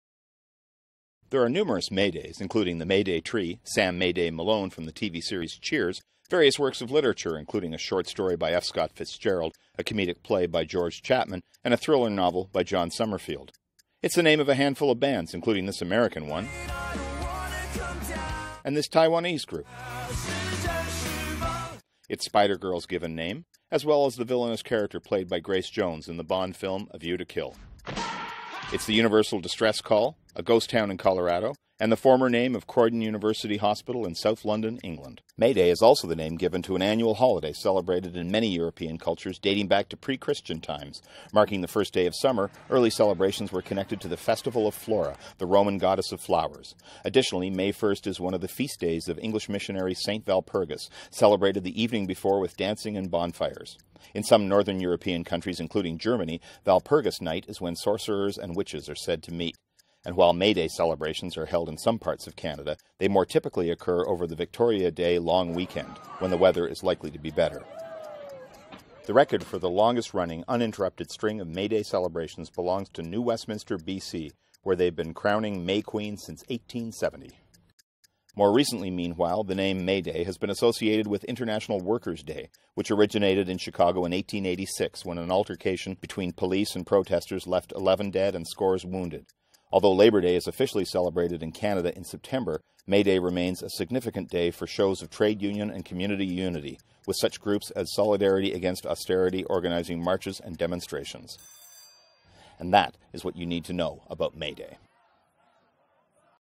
May Day Explainer
In 100 seconds, Bruce Deachman explains some of the history and customs surrounding May Day.